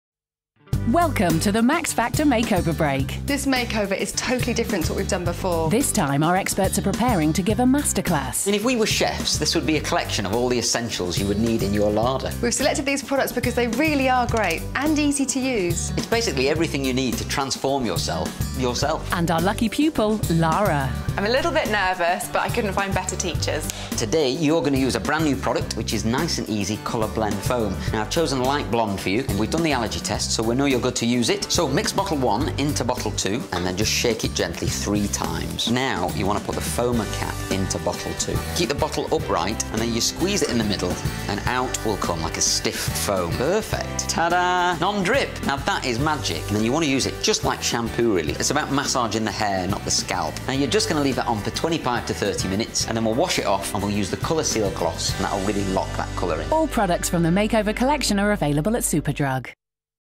Max Factor and Clairol Nice 'N Easy Makeover Break | Transform Yourself - Part 1
Max Factor and Caroline Barnes team up with Clairol to give a lucky lady a makeover break! Watch celebrity hairstylist Michael Douglas show how easy it is to change your hair colour at home with Clairol Nice 'N Easy Colour Blend Foam for a new look! Transform yourself, yourself!

This is a Max Factor makeover break tutorial with Clairol Nice 'N Easy Colour Blend Foam.